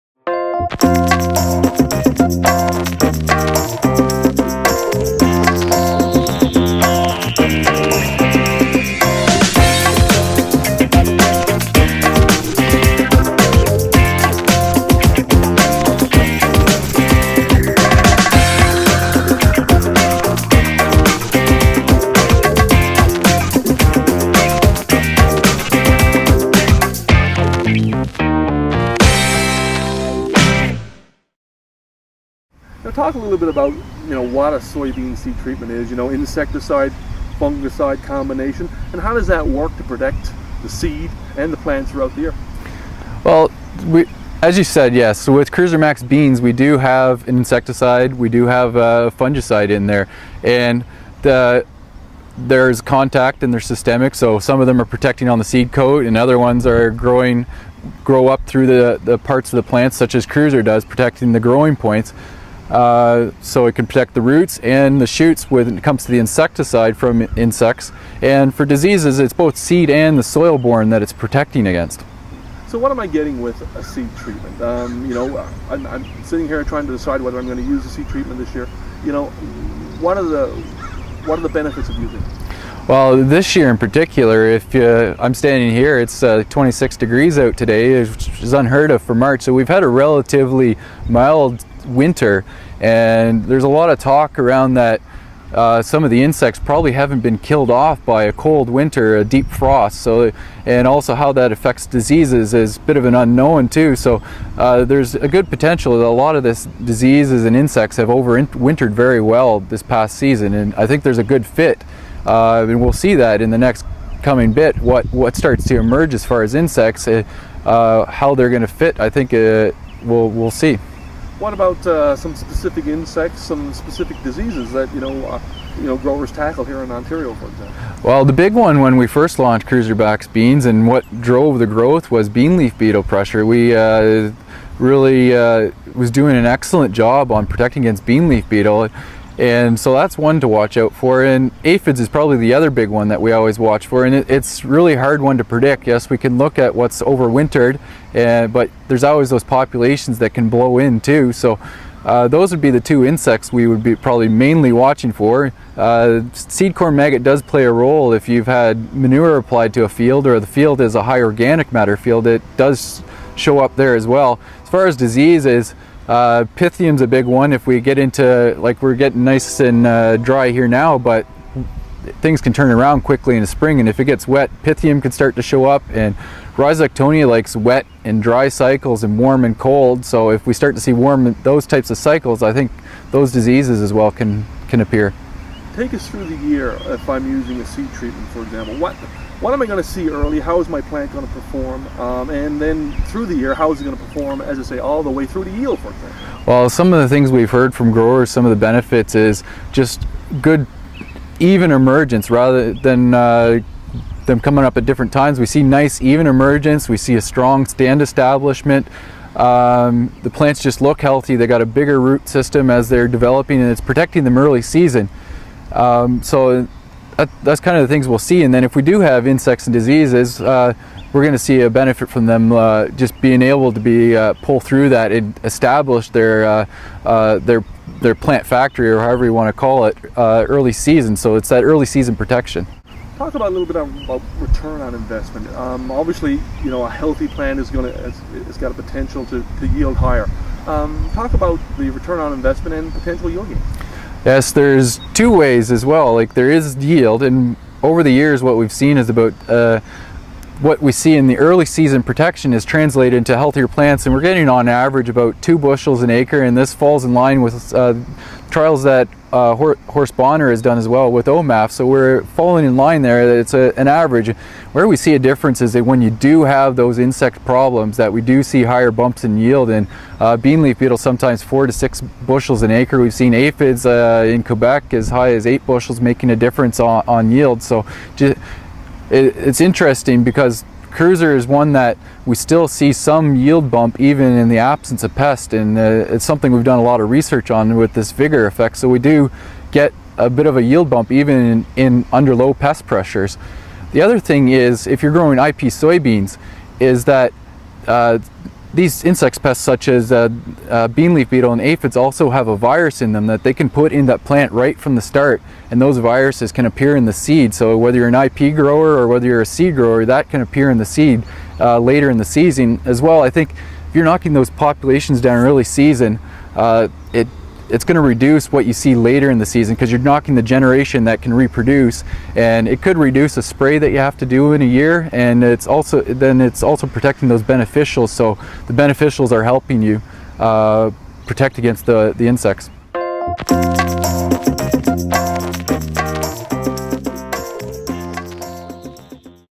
Soybean School - Gaining the Advantage with Seed Treatments
talks to Chris Denys of Syngenta Canada about how seed treatments in soybeans work and the advantages they bring.